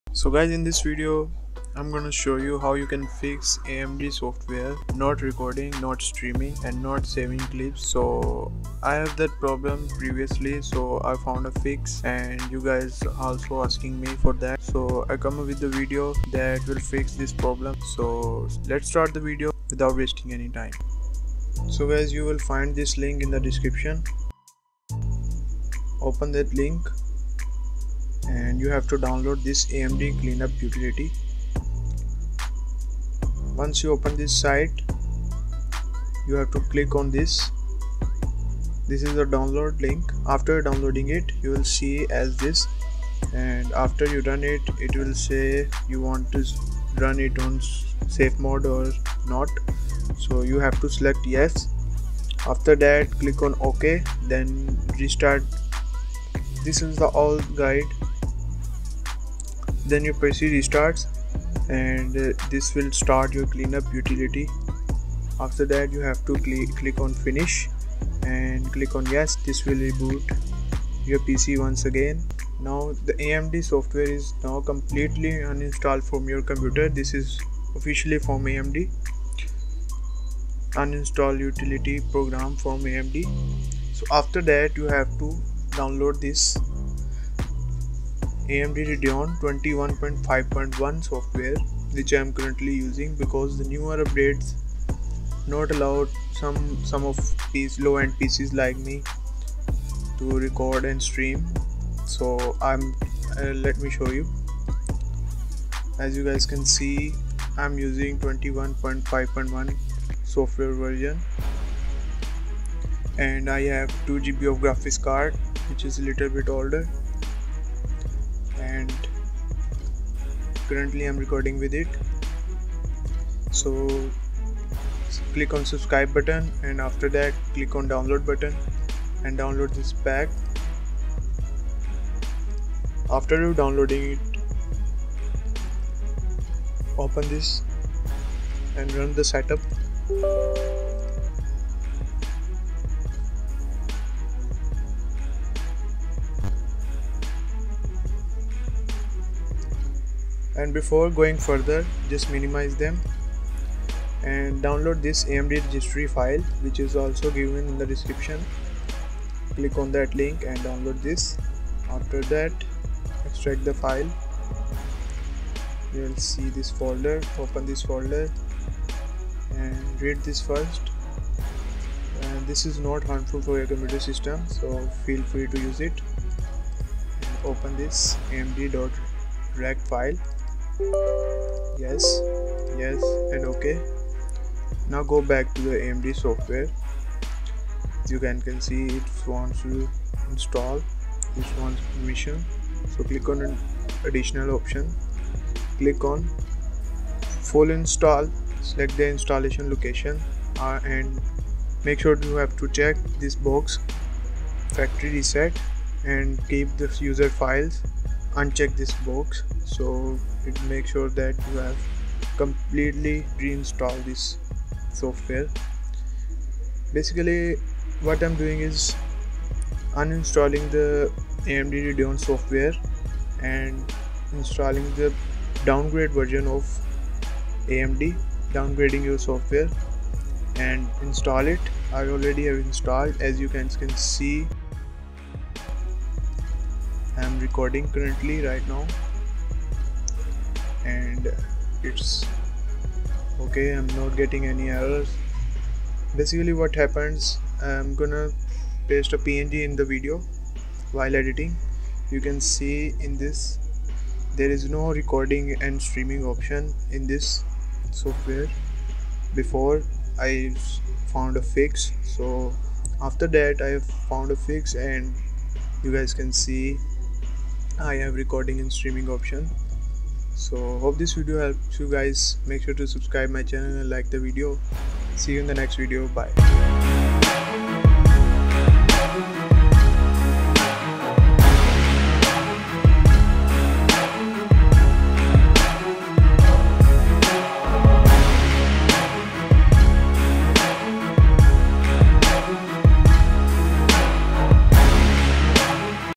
AMD RADEON SOFTWARE : How To Fix Recording / Streaming and Instant Replay - After Update!!
▶AMD RADEON Recording And Streaming and Instant Replay not working or not showing - After Update!!
RAM:   8 GB ddr3 1600 MHz
Windows: 10 pro 64 bit
GPU:   R9 270 2gb AMD Dataland
Moniter: 60 Hz
Power supply: 500 mw